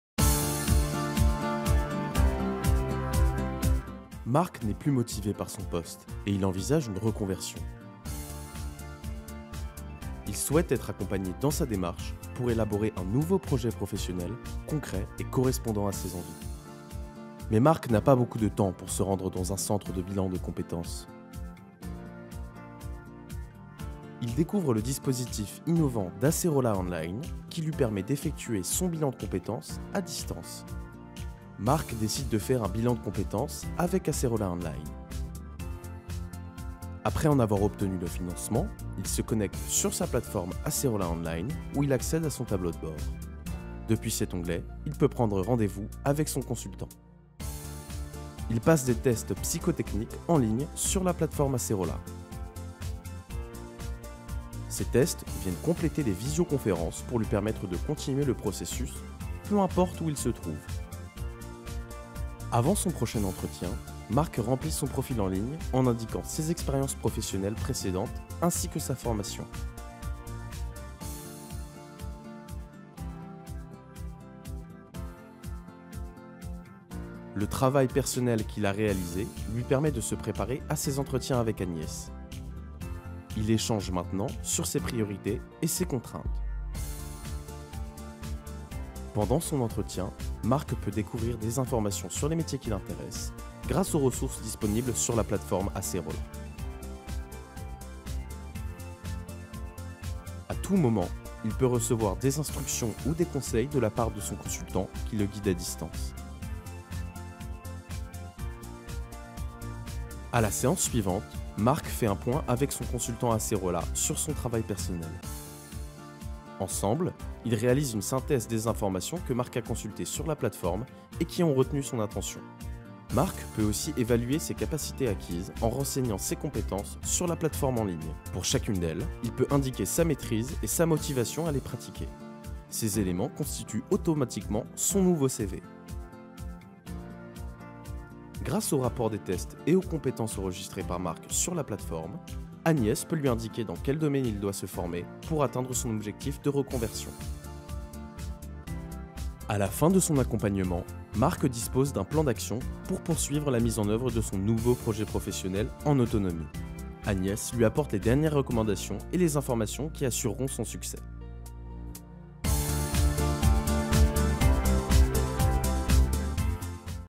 Bilan de compétences en ligne
Acérola Online met à la disposition de ses clients une plateforme de bilan de compétences en ligne. Ainsi nous apportons des solutions d’accompagnement aux transitions professionnelles pour les salariés résidant à l’étranger ou éloignés des grands centres urbains. Acérola Online offre la possibilité à chacun partout en France et à l’étranger de réaliser une démarche complète de bilan de compétences, sans avoir à se déplacer. Utile pour les salariés à l’agenda chargé, les professionnels itinérants, c’est aussi la solution pour tous ceux qui souffrent d’un handicap ou d’une maladie qui restreint leur mobilité.